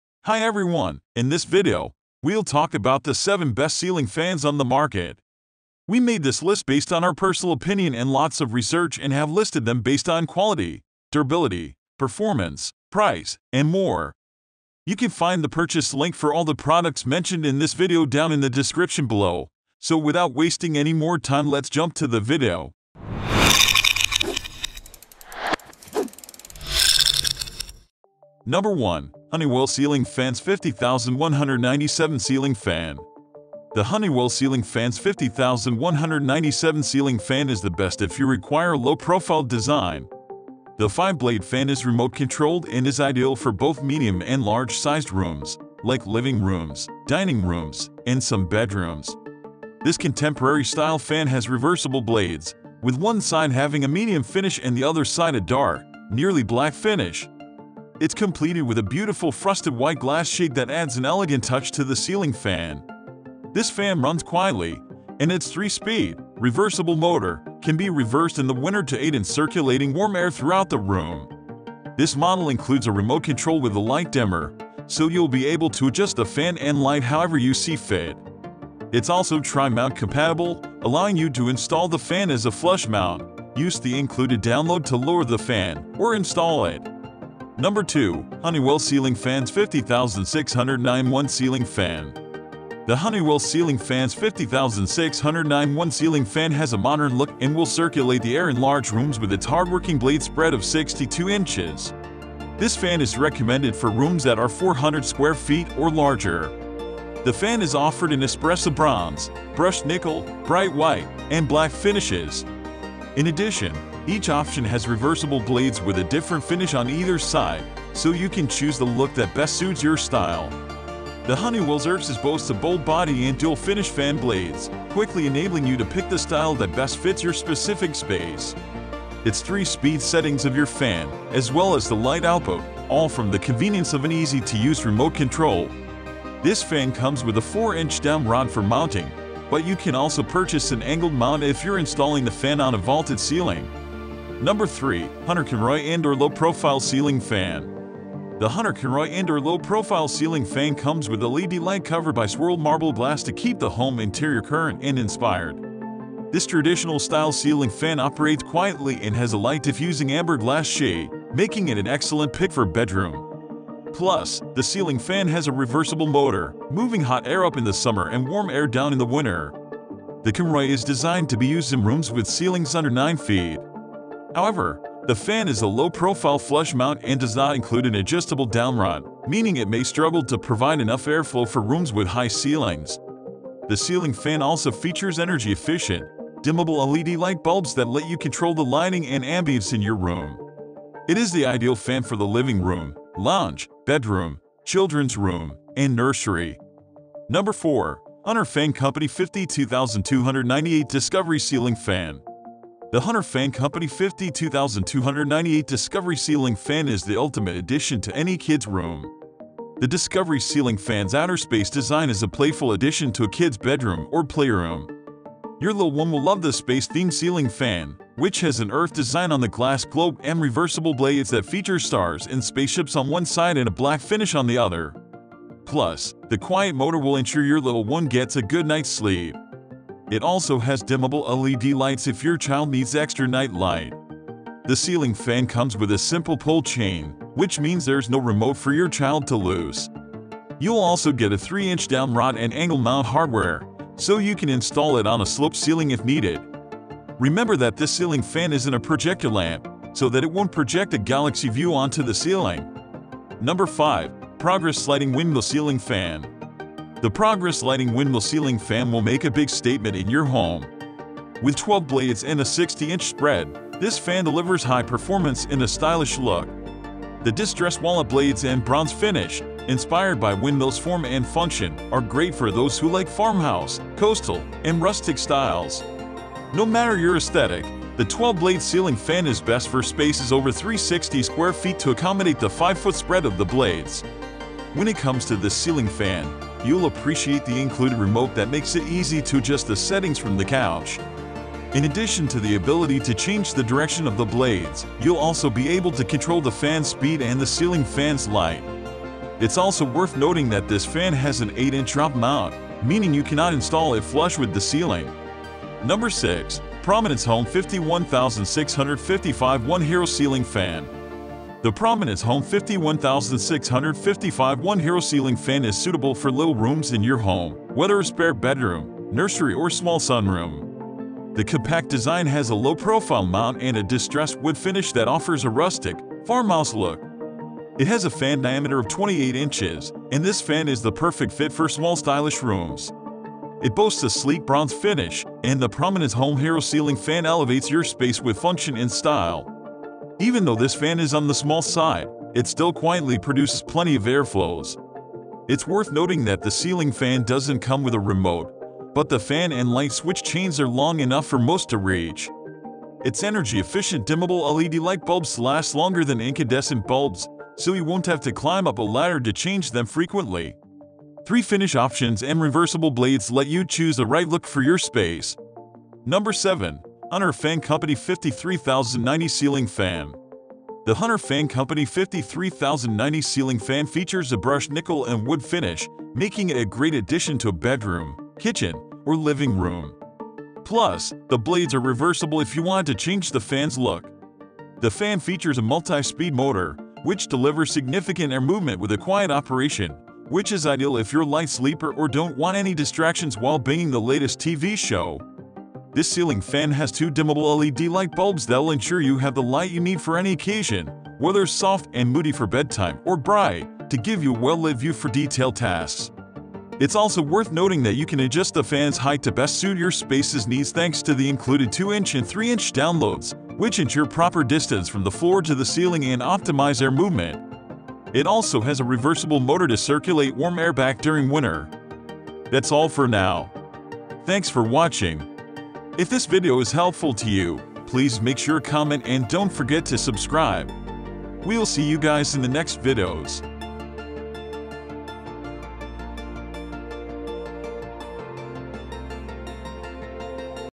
Best Ceiling Fans 2023 | Top 7 Best Ceiling Fans Reviews
In this video, we are going to reveal the Best Ceiling Fans on Amazon of 2023

1. Honeywell Ceiling Fans 50197 Ceiling Fan

2. Honeywell Ceiling Fans 50609-01 Ceiling Fan

3. Hunter Conroy Indoor Low Profile Ceiling Fan

4. Hunter Fan Company 52298 Discovery Ceiling Fan

5. Progress Lighting Windmill Ceiling Fan

6. Prominence Home 51655-01 Hero Ceiling Fan

7. Hunter Fan Company 53090 Ceiling Fan

Sometimes, we use some promotional and educational footage in our content.